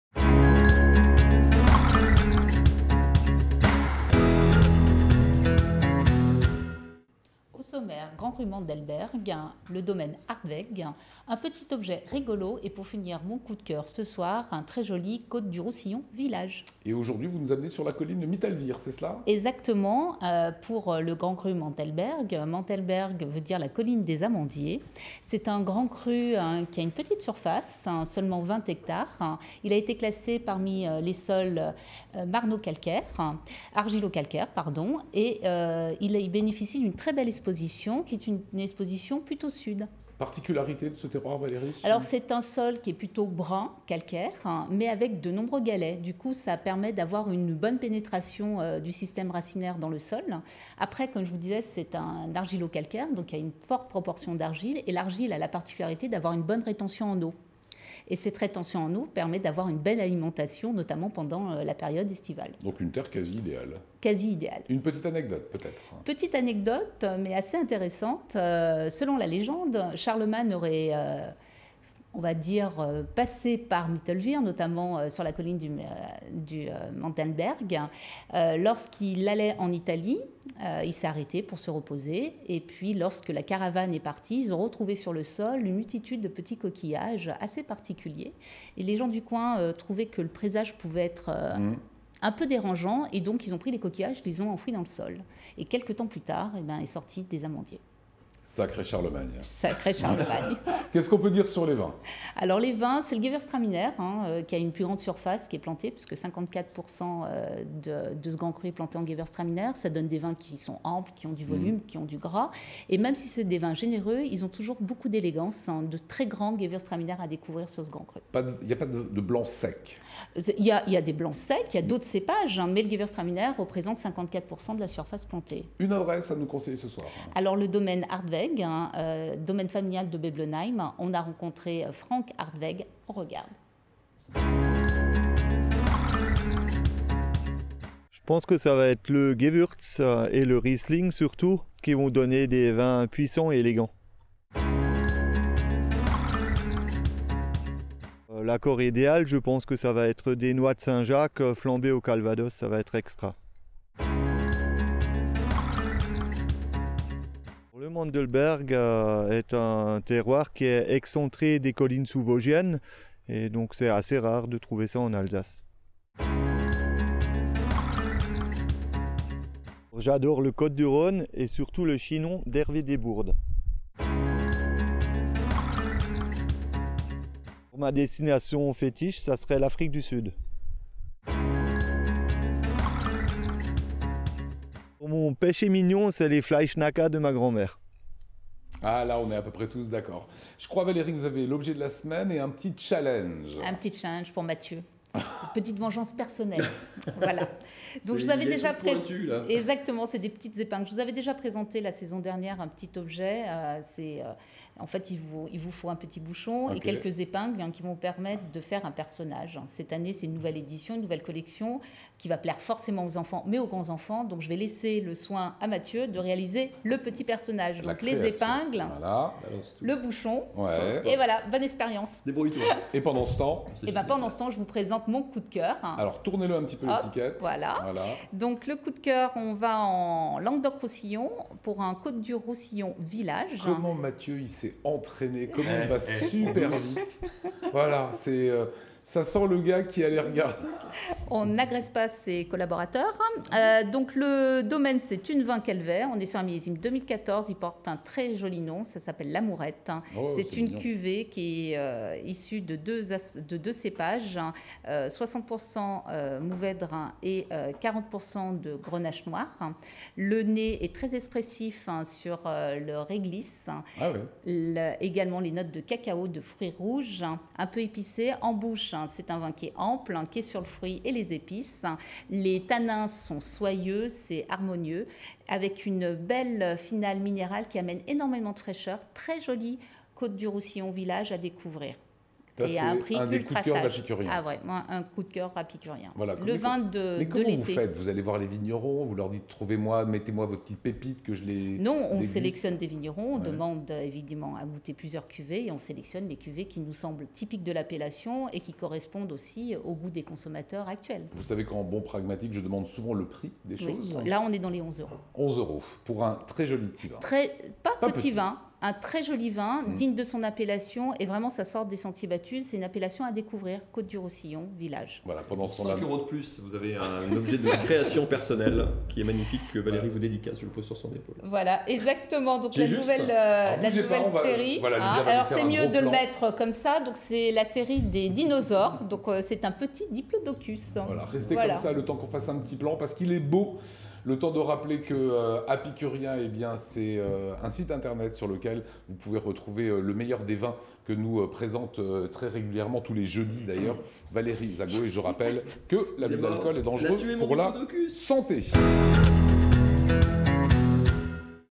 Valérie Zago nous présente le Grand Cru Mandelberg et le domaine Hartweg.
Au sommaire, le grand cru Mandelberg, le domaine Hartweg, un petit objet rigolo et pour finir un très joli vin, un côte du rousillon villages.